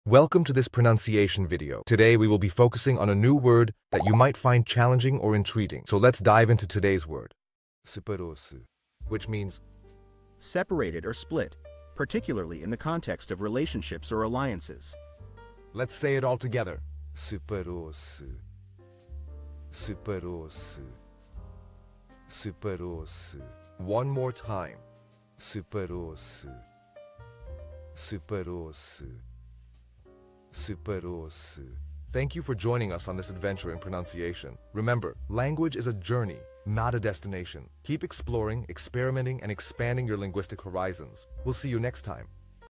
How to pronounce Separou-se
Master the Pronunciation of 'Separou-se Which Means separated' - which means : Separate or split, particularly in the context of relationships or alliances. 📝 with @PortuguesePronunciation 🎓🗣️ - [Your Guide to Portuguese]

Learn to pronounce 'Separou-se' perfectly in Portuguese with our easy and engaging tutorial by @PortuguesePronunciation! 📖📽️ In this comprehensive video, we provide you with native speaker examples 🗣️👂 and phonetic breakdowns 🎵 to ensure you get the pronunciation just right.

How to say Separated in Portuguese